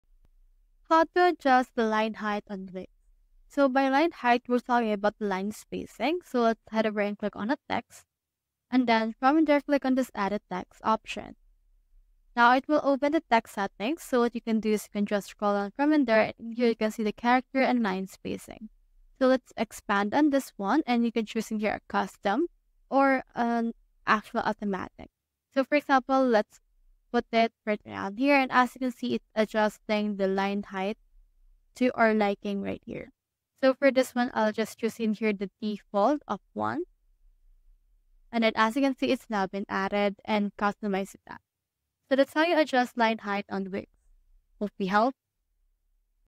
How to Adjust a Line Height on Wix | Wix Tutorial (2025)
Looking to improve text readability on your Wix site? This tutorial will show you how to adjust line height for text, allowing you to control spacing for a cleaner look. I’ll guide you through simple steps to achieve the perfect line height.

In this tutorial, you’ll learn:
How to find and adjust line height settings in Wix.
Step-by-step instructions for adjusting text spacing.
Tips for improving readability and aesthetics.

Make your text easier to read—follow these steps to adjust line height on Wix!

00:00:00 Introduction
00:00:10 How to Add and Edit Text
00:00:20 Adjust Line Spacing
00:00:30 Line Height Preferences